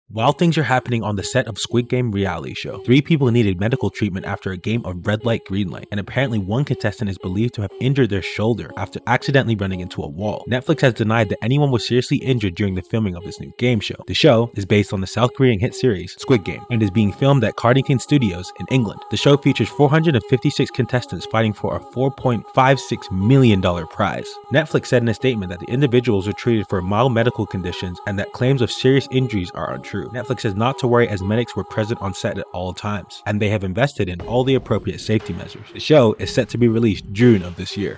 Squid Game Reality Show: Multiple Players Injured
Netflix’s real-life version of Squid Game have spoken out about conditions on the set, with several reportedly leaving in tears and one taken out on a stretcher.

The platform is currently filming a game show in the UK based on the smash hit Korean drama, about desperate members of the public taking part in a deadly competition.

Participants in this real-life, non-fatal version are competing for $4.65 million, the largest lumpsum jackpot in the history of reality TV.

According to The Sun, contestants spent several hours in freezing temperatures of -3C, with the rules of one game, “Red Light, Green Light”, demanding that players stand completely still.

One player told the publication: “Even if hypothermia kicked in, then people were willing to stay for as long as possible because a lot of money was on the line. Too many were determined not to move so they stood there for far too long.

“It was like a warzone. People were getting carried out by medics but we couldn’t say anything. If you talk then you’re out.

“Some people couldn’t move their feet because it was so cold.

“You could hear someone yell ‘medic’ and the crew would rush on. We ended up standing there for 30 minutes between takes. Some were crawling by the end. At least one was carried out on a stretcher.”